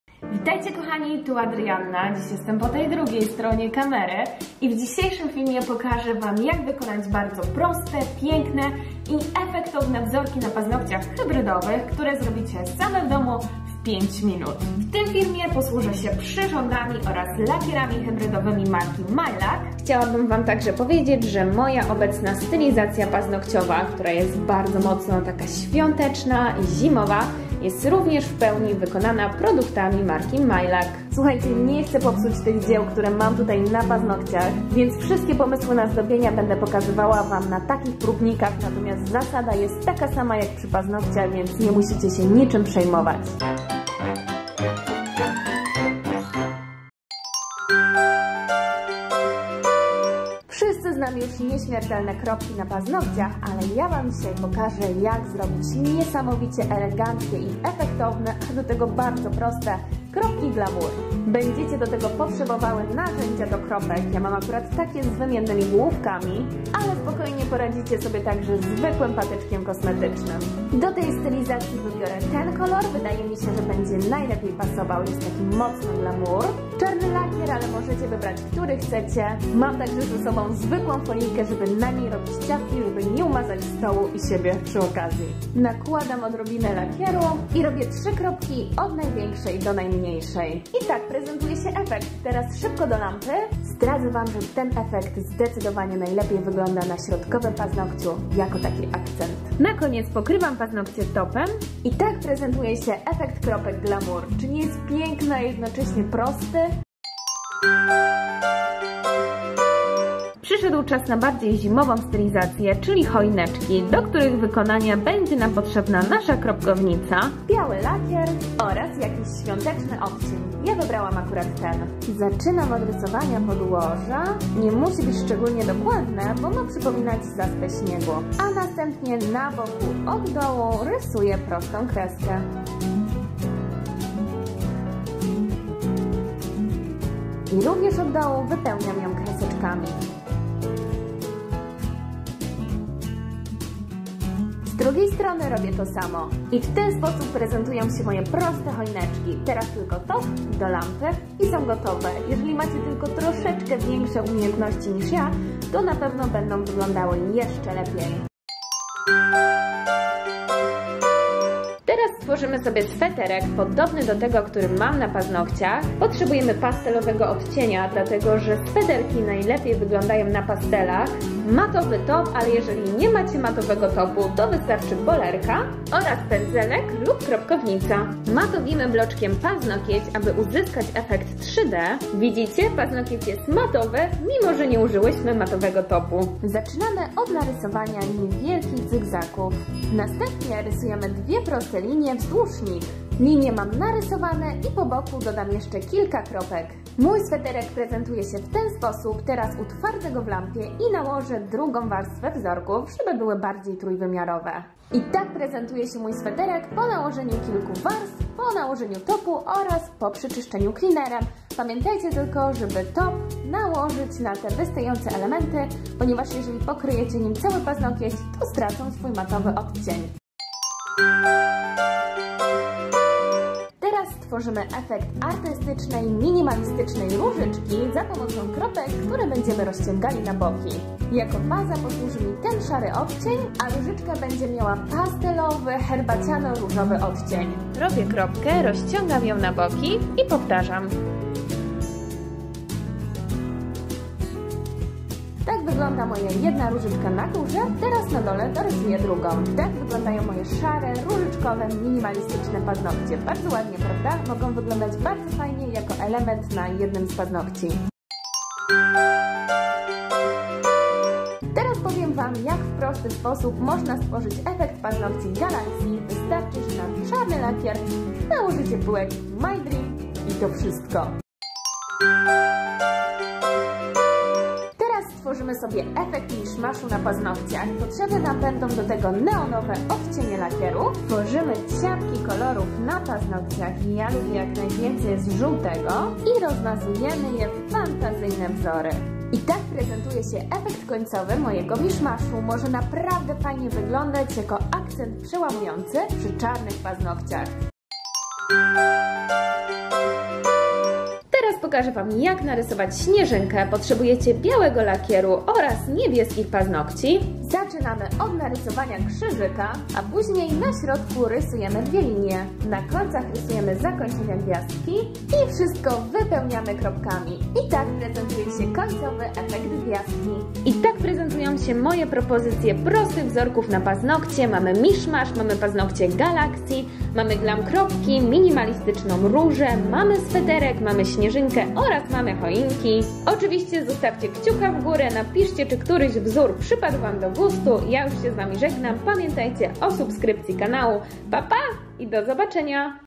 Proste i szybkie wzorki na paznokcie, które zrobi KAŻDY ✨
Film powstał we współpracy z marką Mylaq 🦄🦄🦄

Kochani! 🦄 W dzisiejszym odcinku pokażę Wam kilka prostych i szybkich wzorków na paznokcie hybrydowe, które zrobi każdy! 🦄